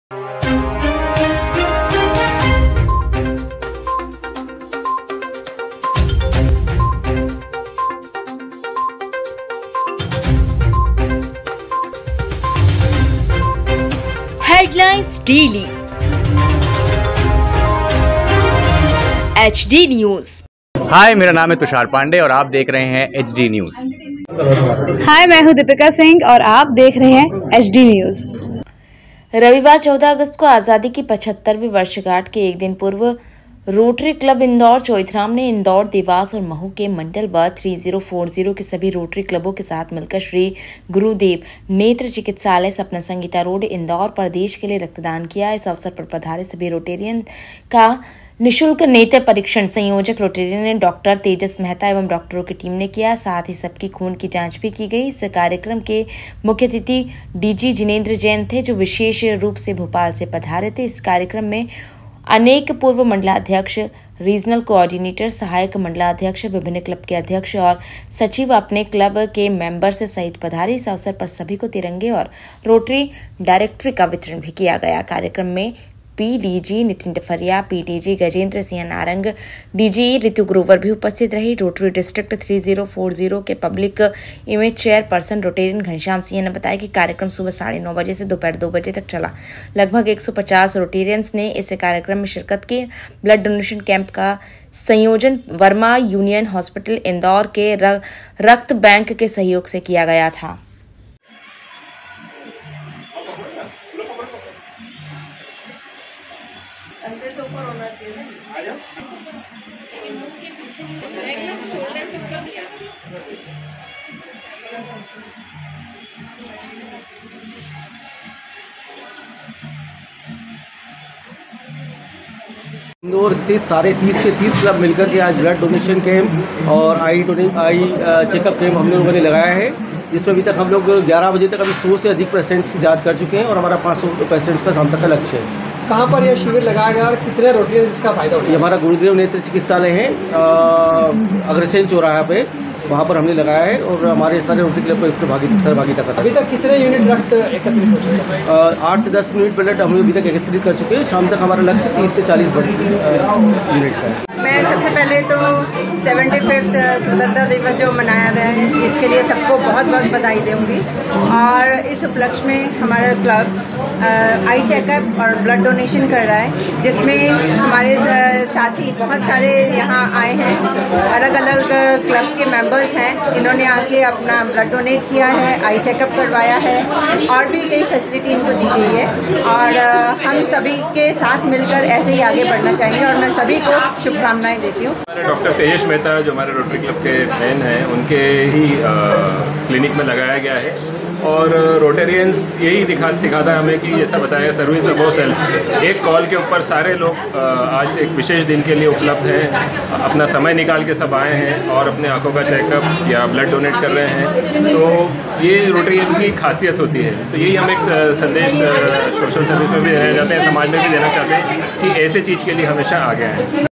रोटरी क्लब द्वारा आजादी की 75 वीं वर्षगांठ पर अमृत महोत्सव के अंतर्गत रक्तदान शिविर एवं निशुल्क
नेत्र एवं रक्त परीक्षण शिविर का आयोजन संपन्न.. रविवार को इंदौर में श्री गुरुदेव नेत्र चिकित्सालय सपना संगीता रोड पर हुआ आयोजन